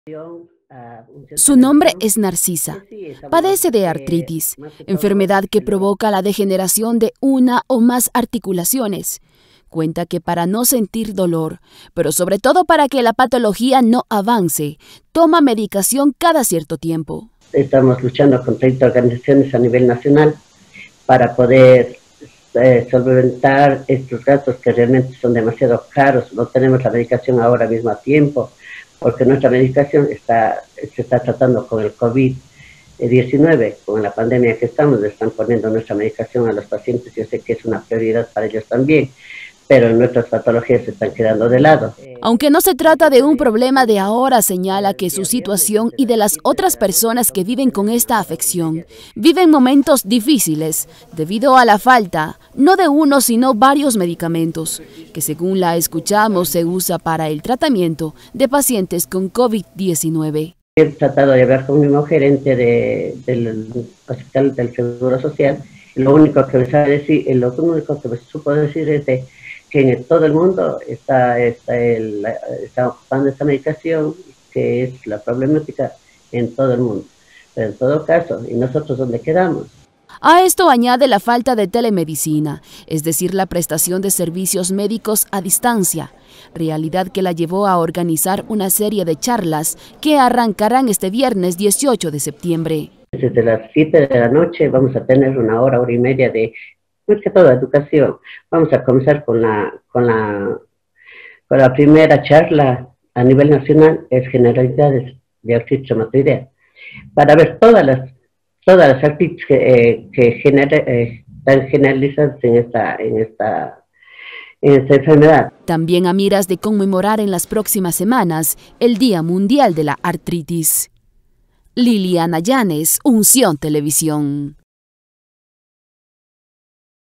Pacientes con artritis atraviesan duros momentos debido a la covid-19
Desde marzo a la fecha, con los primeros casos de covid-19 en el país; los pacientes que padecen artritis atraviesan momentos difíciles ante la falta de medicación para el tratamiento de su enfermedad. Hacen un llamado a las autoridades de salud para encontrar soluciones a su realidad.